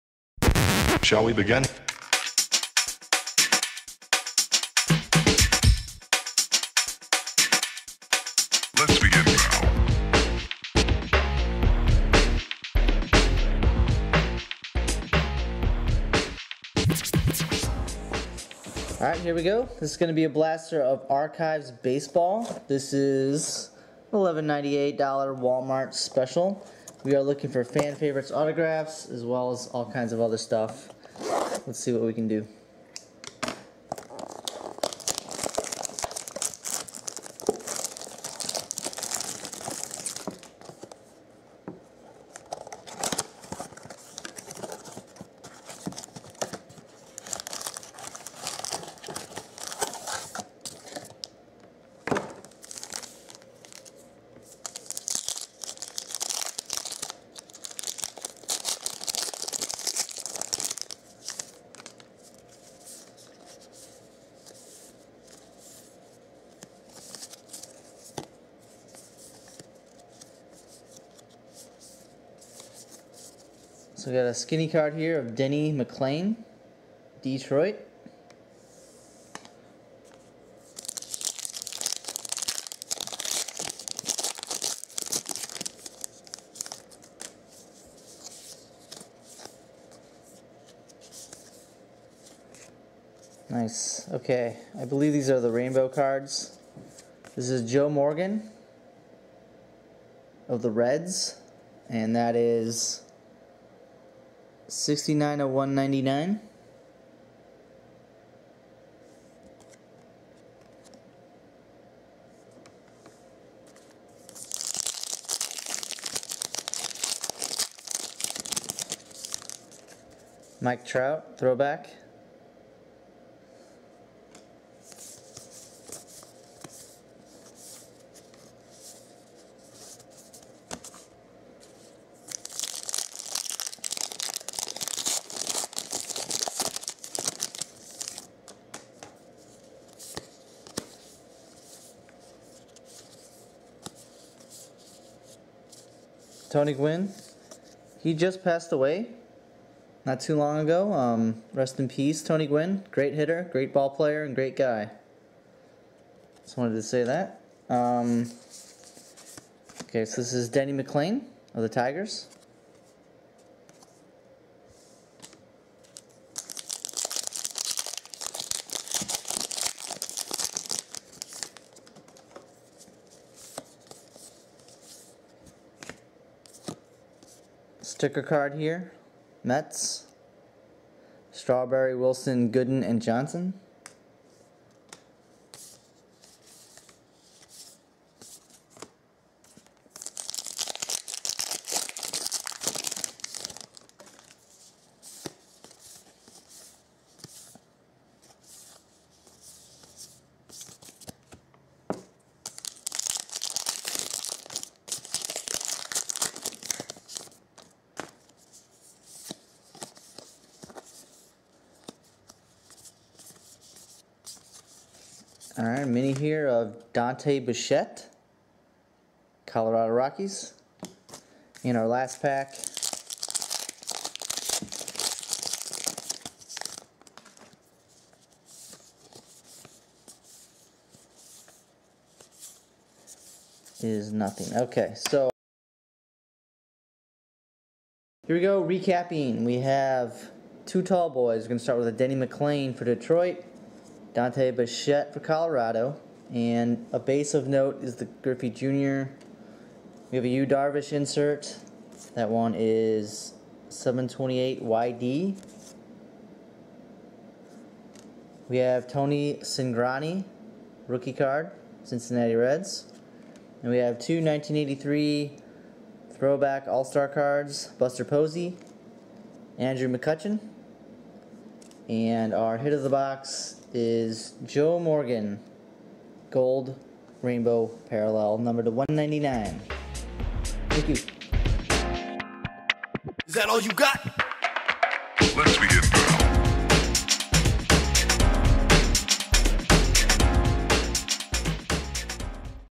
2013 Archives Baseball Blaster Box Break
Trying out a blaster box from the local big-box store.

Baseball player hitting ball intro video: Getty Images/Vetta
Recap video, sports balls (looped): Getty Images/iStockfootage